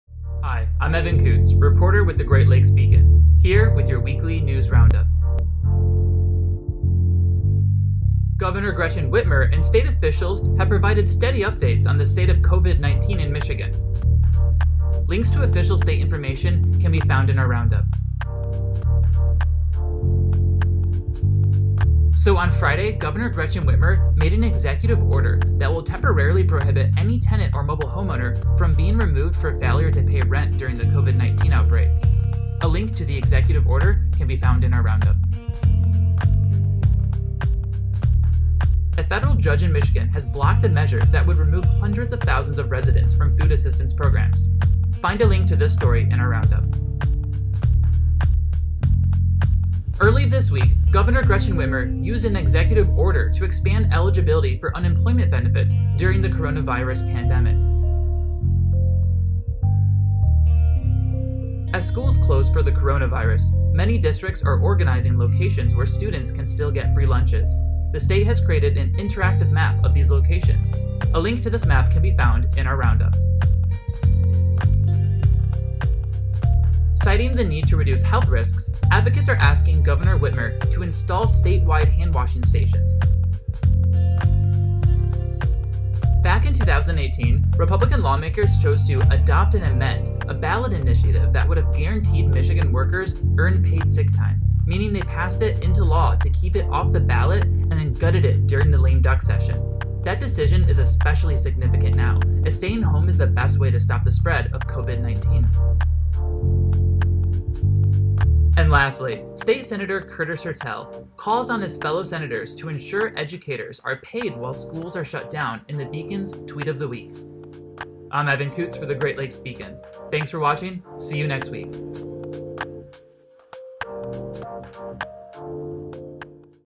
Navigating the Week: GLB's Weekly Roundup 03/20
There's a lot of news this week in Michigan surrounding coronavirus. Catch up with the Beacon's Weekly Roundup